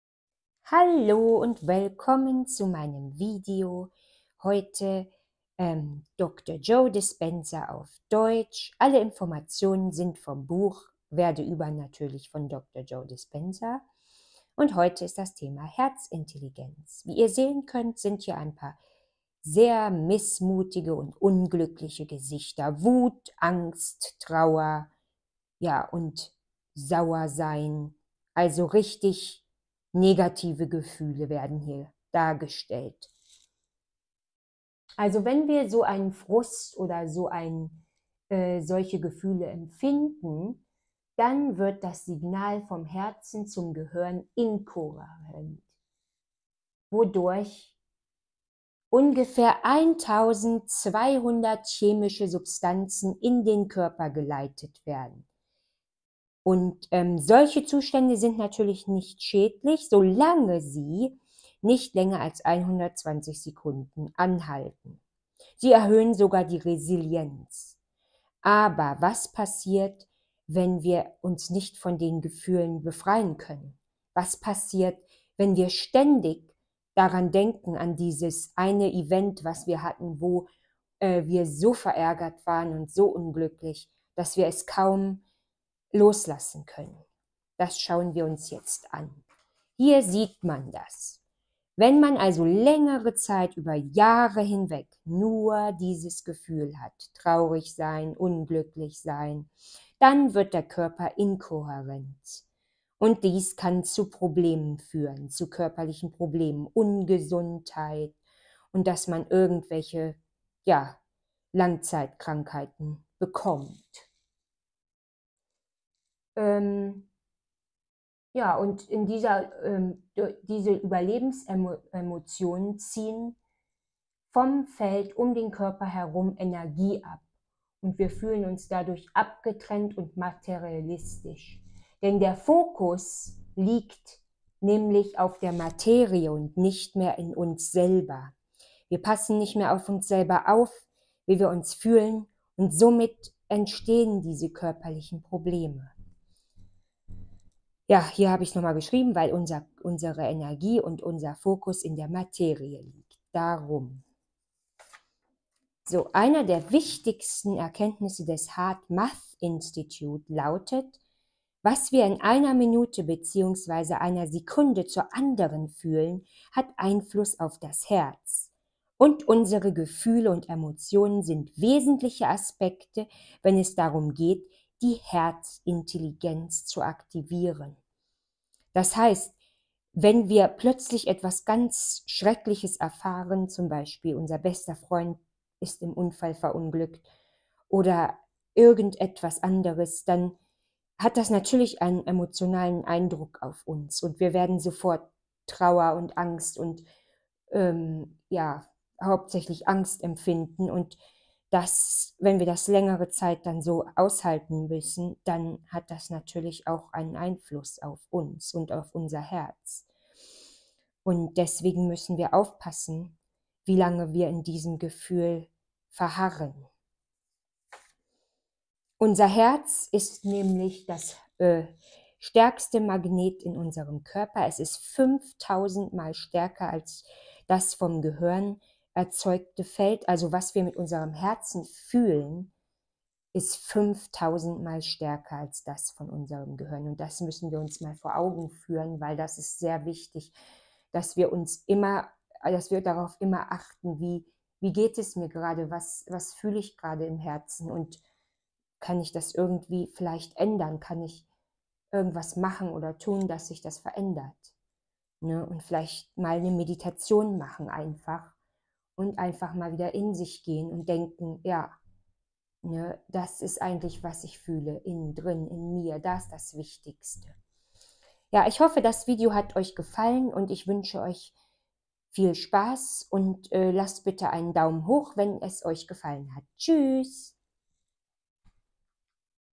Dr. Joe Dispenza auf deutsch: Herzintelligenz
Wie stark ist das Herz? Ist es wichtig wie wir unsere Gefühle verändern?